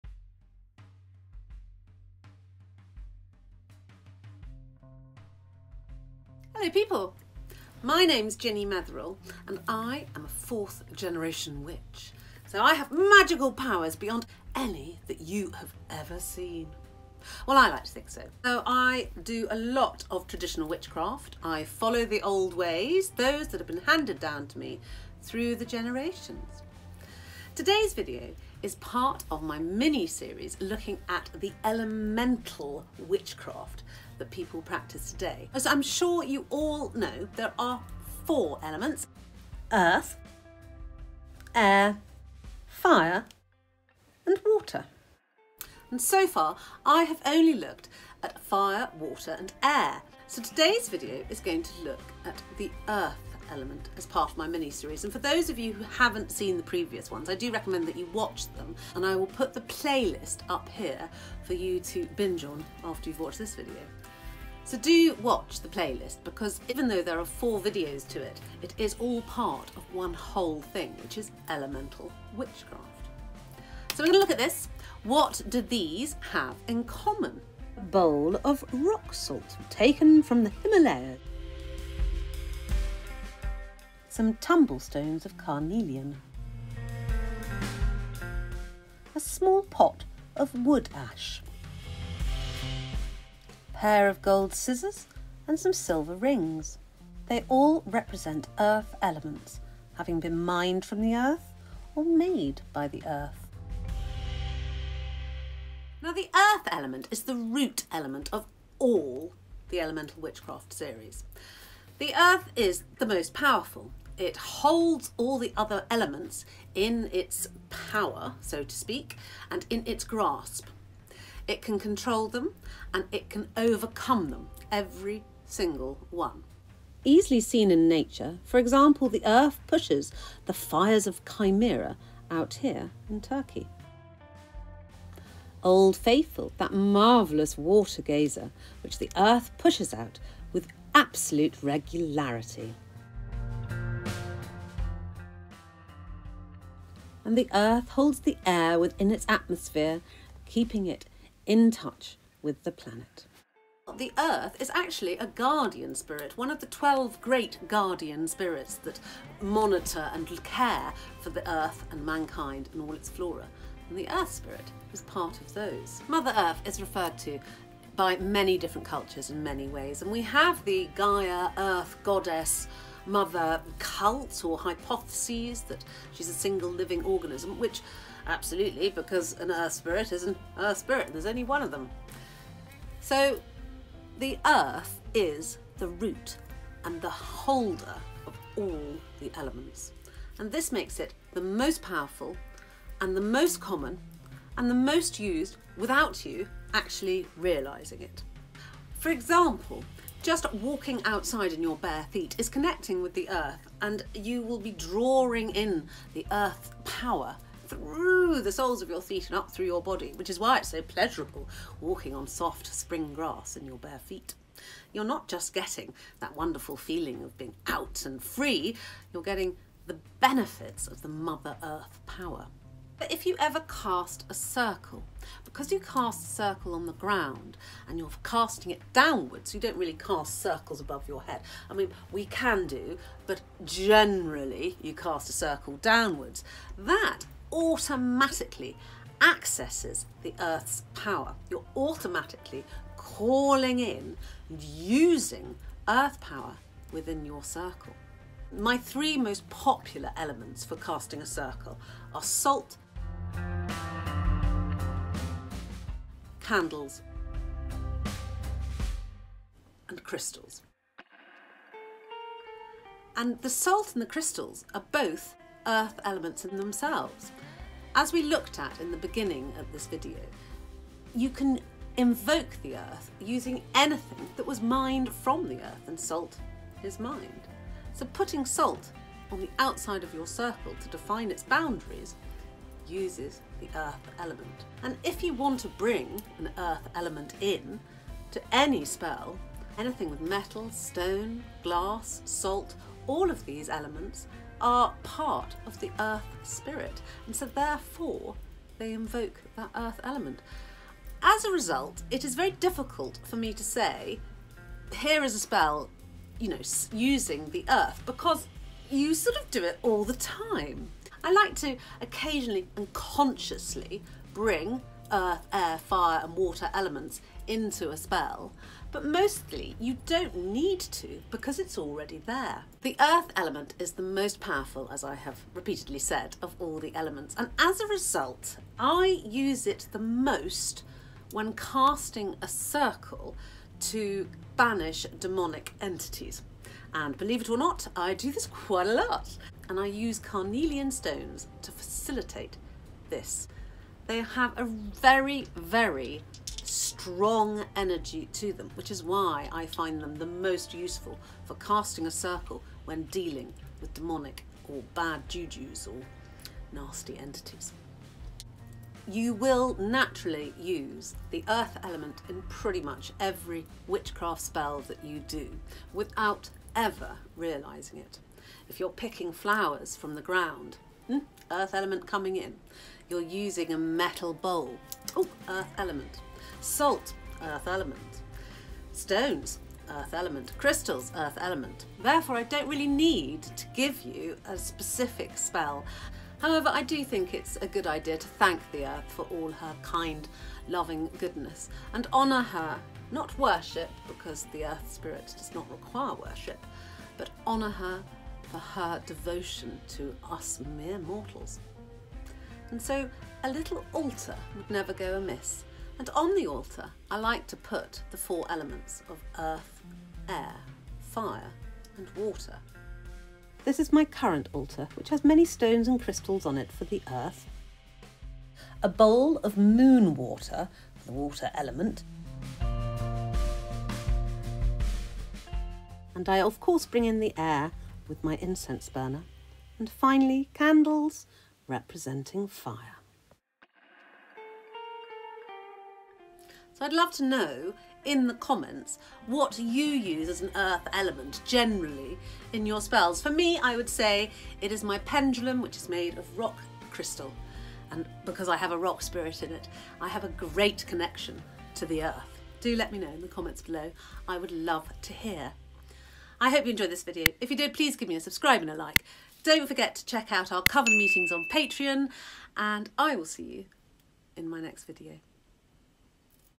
Witchcraft & Earth Magick || Elemental Witch Series
Finally - after a prompt from a commenter, I have completed my elemental witch series with this video on Witchcraft & Earth Magick.  Earth is the root of all the elementals that you would use in your everyday and high days magic.  I try and explain how the earth is used, as it is a great grounding and stabilising element.  It also controls all the other elements and therefore is the ‘grand high magic’ in the elements.

I try and show how earth magick is used by me in my witchcraft, and how often people use it without realising.  Remember that phrase ‘touch wood’. That will be using the earth magic!  Earth magic is definitely my favourite element, because of the immense magical power and safekeeping it gives.

My elemental witch series should be taken as a whole, as each element relies on the other, so here is the playlist do watch it and let me know your thoughts - which is your favourite element to use.

I would love to hear from you so please keep in touch x